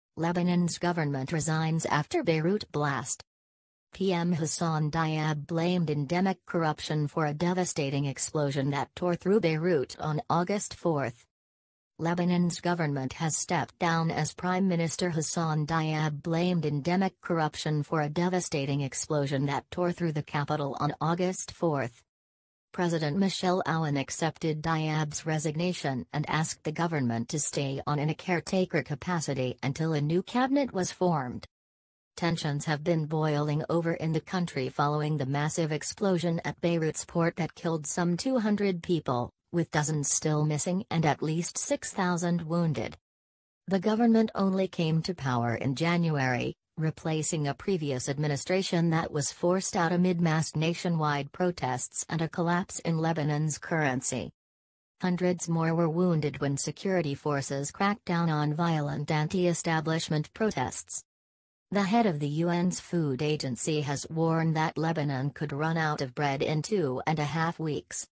Lebanon's government resigns after Beirut blast
PM Hassan Diab blamed endemic corruption for a devastating explosion that tore through Beirut on August 4.

Lebanon's government has stepped down as Prime Minister Hassan Diab blamed endemic corruption for a devastating explosion that tore through the capital on August 4.

President Michel Aoun accepted Diab's resignation and asked the government to stay on in a caretaker capacity until a new cabinet was formed.

Tensions have been boiling over in the country following the massive explosion at Beirut's port that killed some 200 people, with dozens still missing and at least 6,000 wounded.

The government only came to power in January, replacing a previous administration that was forced out amid mass nationwide protests and a collapse in Lebanon's currency.

Hundreds more were wounded when security forces cracked down on violent anti-establishment protests.

The head of the UN's food agency has warned that Lebanon could run out of bread in two-and-a-half weeks.